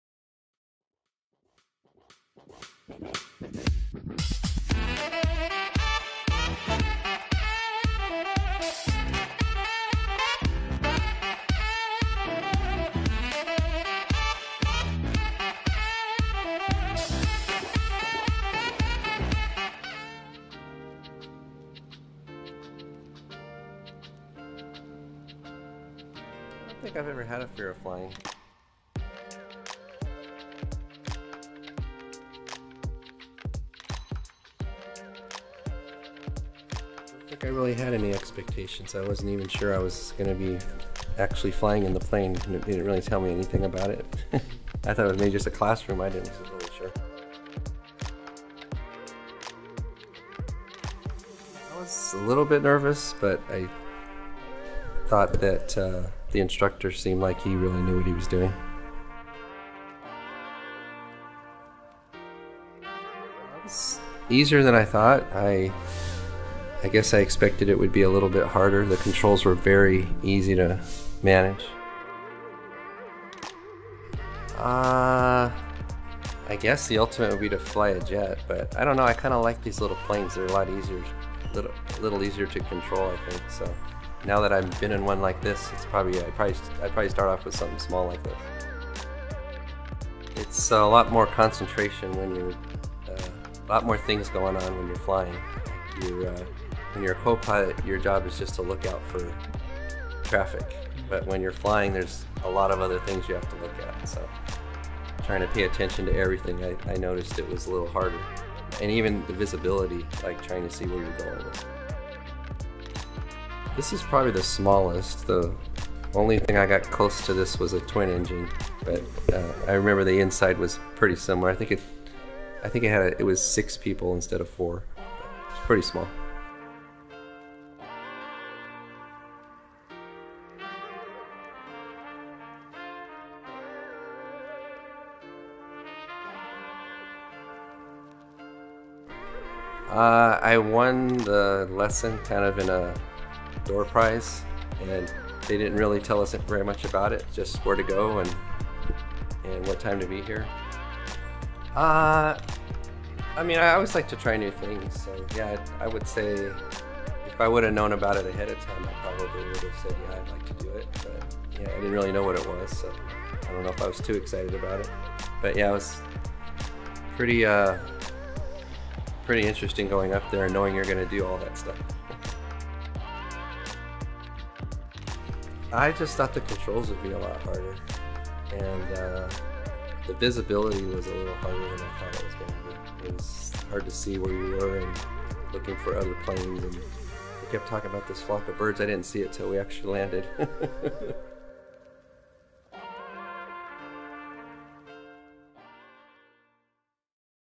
James Maloney Flying with DuBois Aviation
This video is in collaboration with Chino Chamber of Commerce. He's one of four contests that won a flying lesson at DuBois Aviation. We interview Mr. Maloney about his experience flying an entry level airplane.

Editor- Emmanuel Vargas
Camera A/B- Alaast Kamalabadi, Emmanuel Vargas
Sound- Emmanuel Vargas
Writer- Brody D. Salazar.